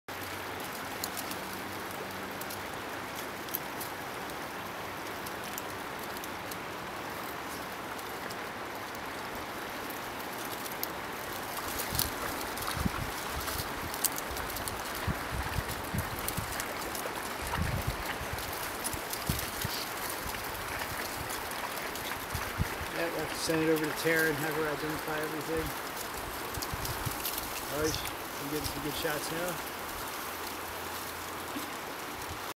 Glacier National Park - Terrestrial Gartersnakes
A mating ball of Terrestrial Gartersnakes (Thamnophis elegans) near a hibernaculum; several males are vying for access to a female. Snakes breed near their overwintering area because males can guard the hibernacula entrance since they know females will be coming to that area from around the landscape.